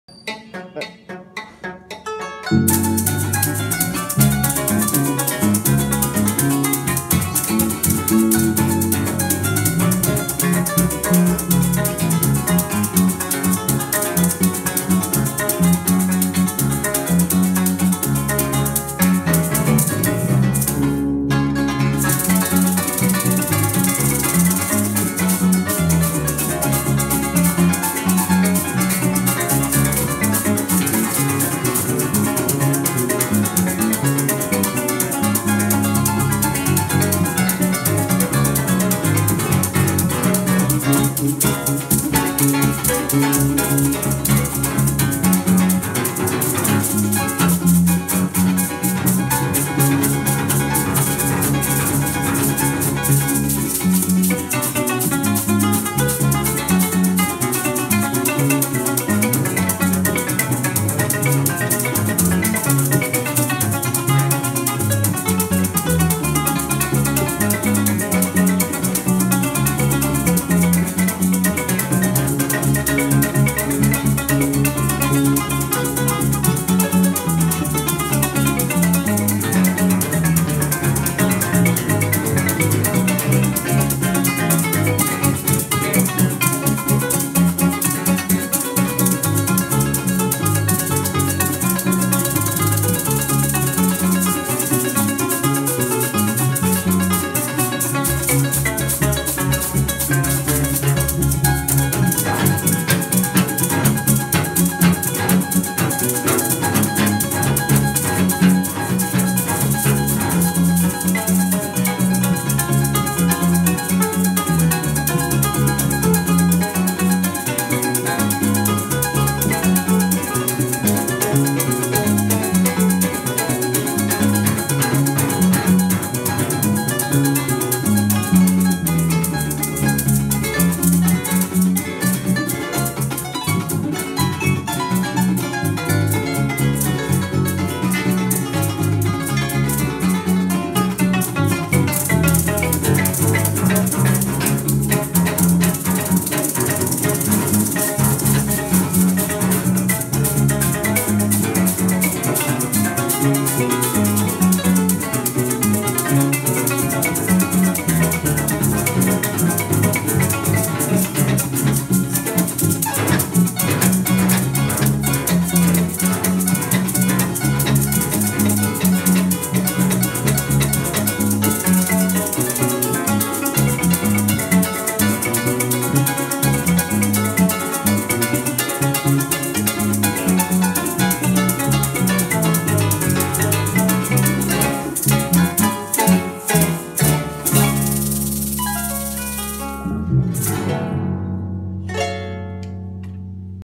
Pieza del folklore Llanero.
otros de los instrumentos que constante mente estudio es la Maraca Venezolana un instrumento que se las trae.

músicos:

Angelis Sala (cuatro)
Elbys Medinas (arpa)
Eudys Colmenares (bajo)
Oscar Adjunta (maracas)

bajo la producción de OAdmusic.
lugar de grabación:
balcón de Tama
grabación y edición: Carlos Mogollón